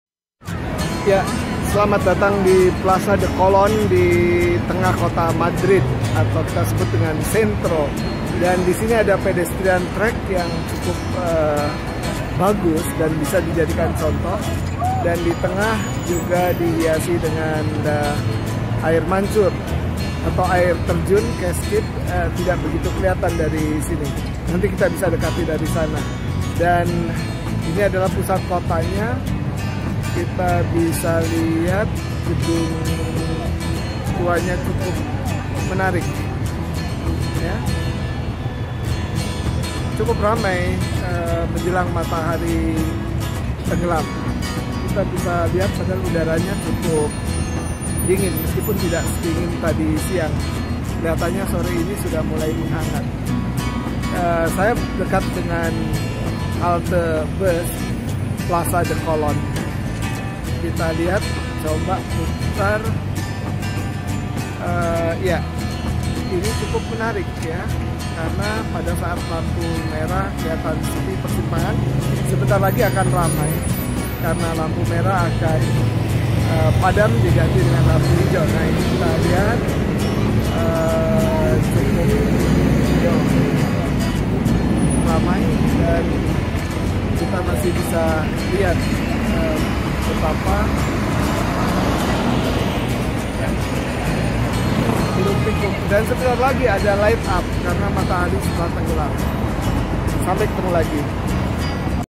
A winter break in Centro Madrid Spain
A winter break in Centro Madrid Spain, traveling edition of Professor Hadi Susilo Arifin "Deddy"